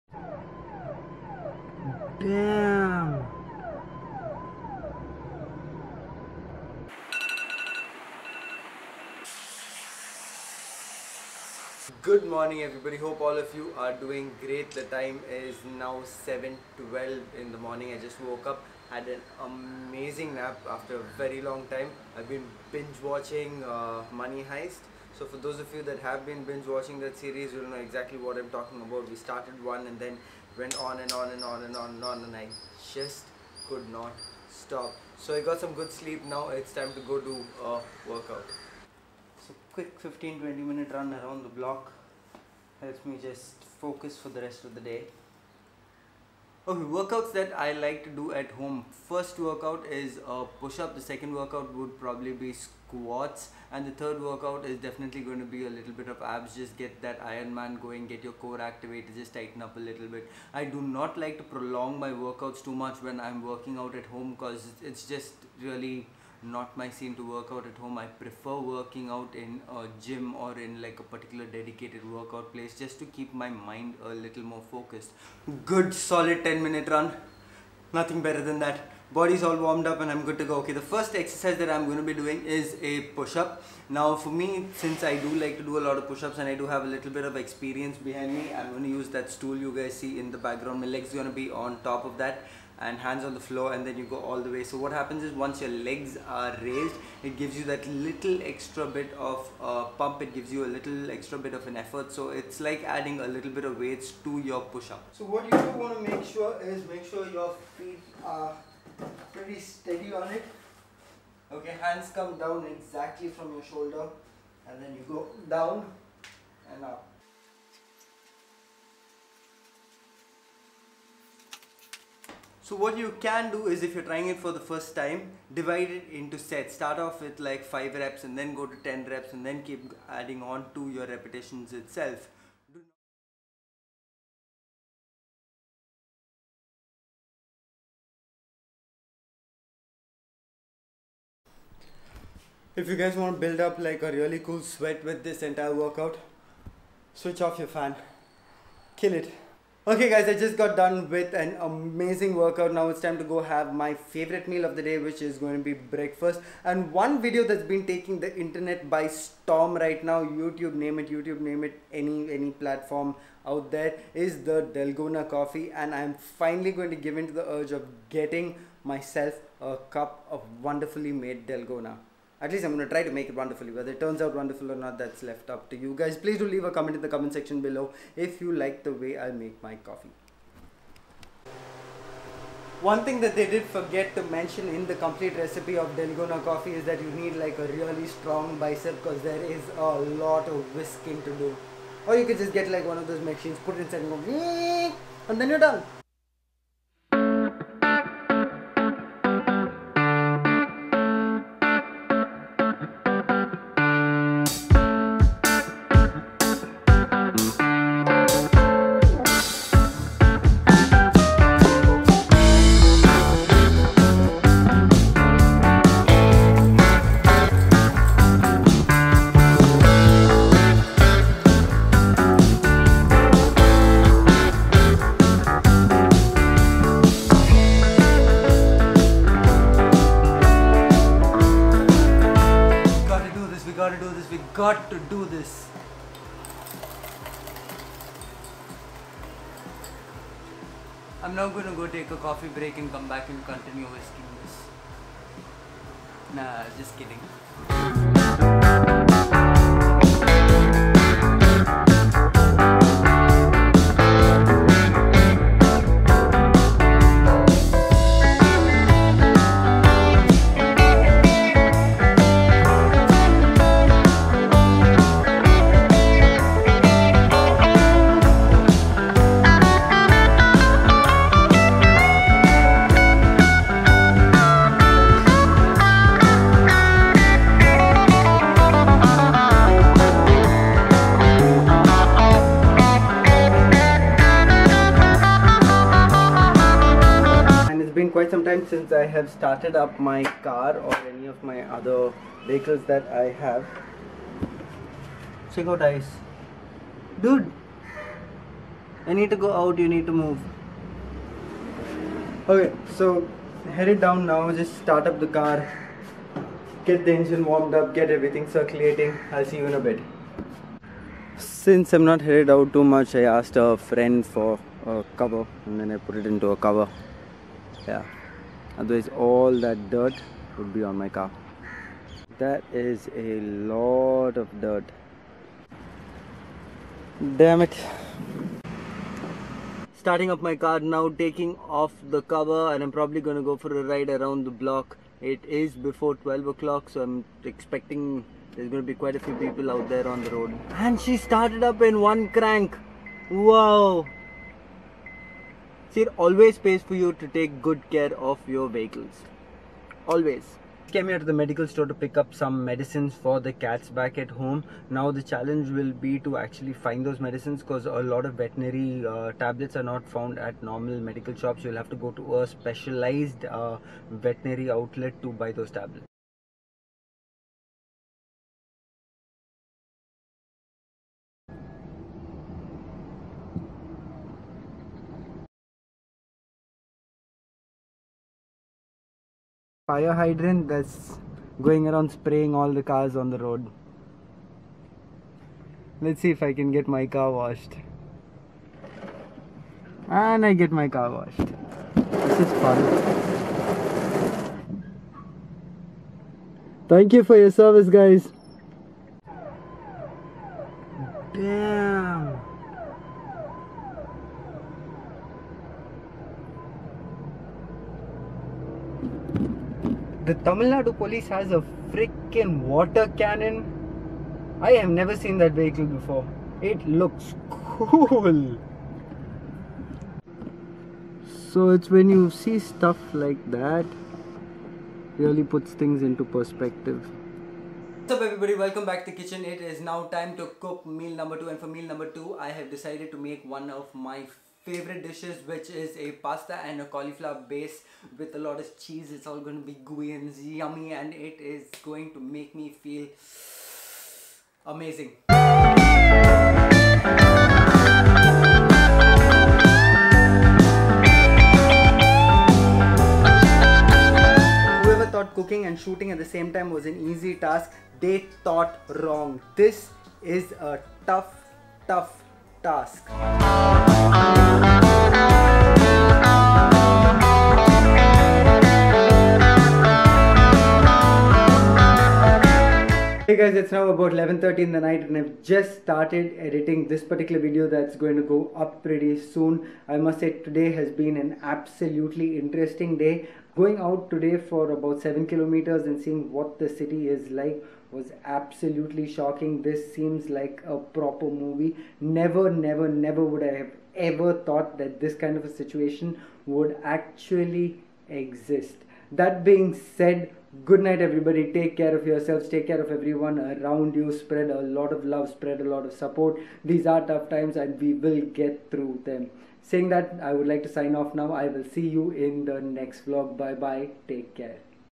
Fire fighters ,Water canons and Dalgona coffee .. ( under quarantine )
Cooking has always been an art that I have loved right from the time I was a kid , Always like to try something new .
I tried my hand out at the Dalgona coffee , loved it .
I had to head out to pick up some medicines for the pets , which meant I had to take a 7km drive to Vepery and what a drive , the experience absolutely surreal .
Major shoutout to all the firefighters and police officers for going beyond the call of duty.

Shop Details ;
No34 , Pets Paradise - open till 2pm